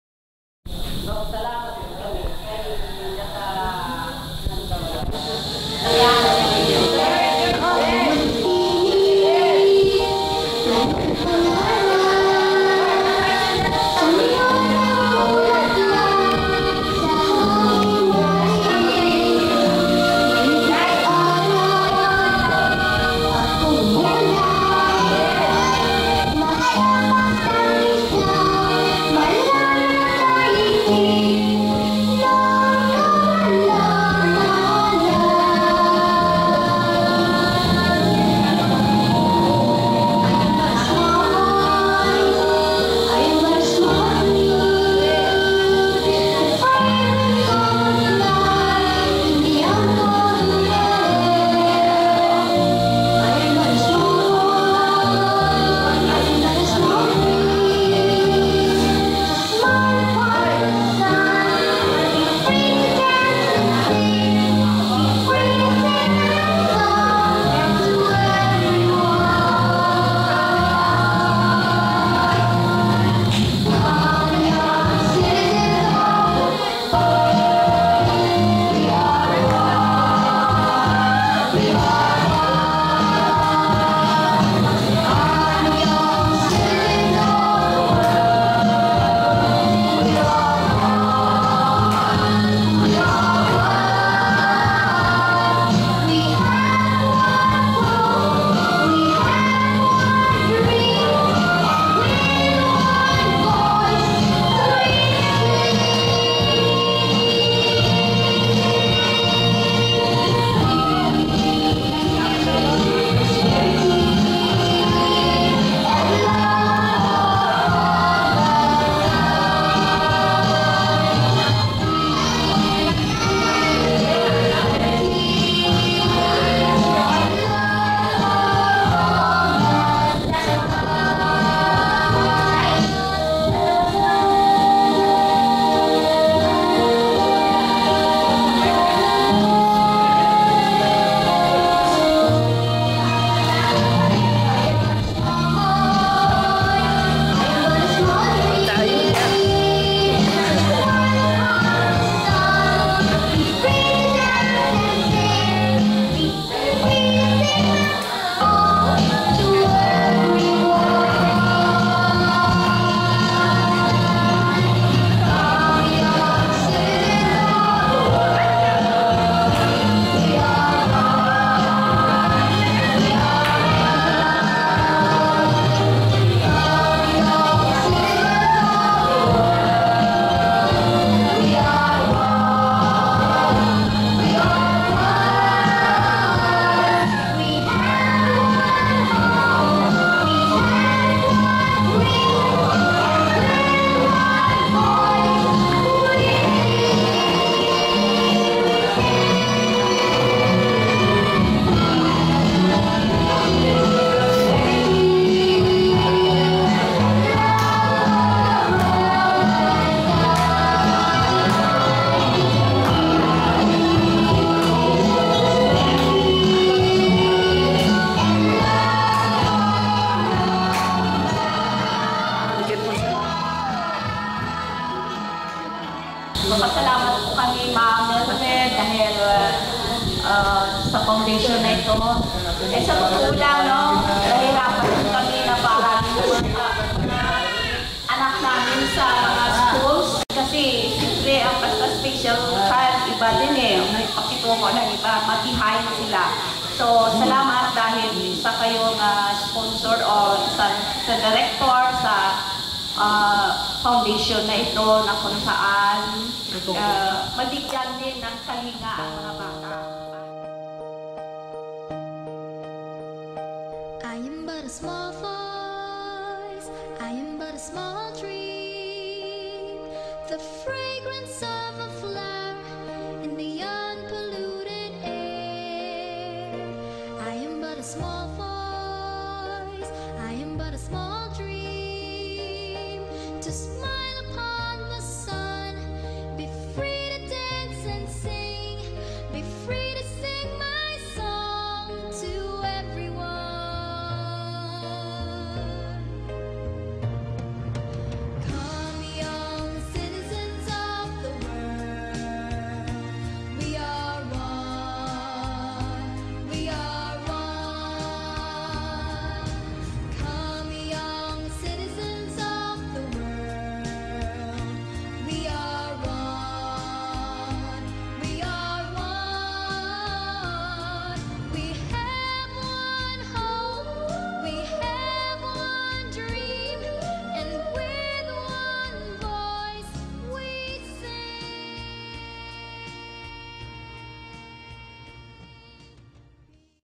Onze school
In het nog niet helemaal afgewerkte activiteiten centrum leren deze kinderen met een beperking sneller dan verwacht. Trots op deze kinderen. HAPPY!